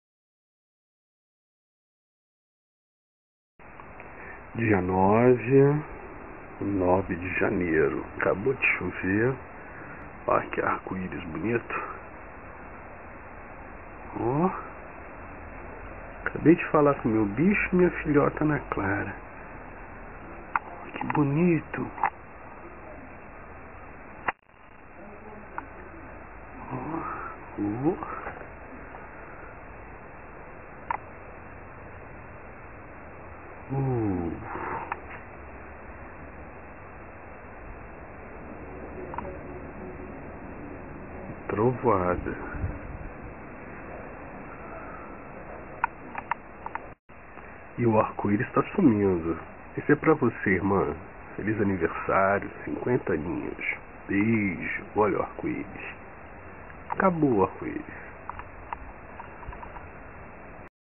Presente pra Denise _ Arco-iris
Olha o seu presente irmã, tinha acabado de ver/faalr co Rosely e Ana Clara e olhei para o cé...bjão! Feçliz Aniversário !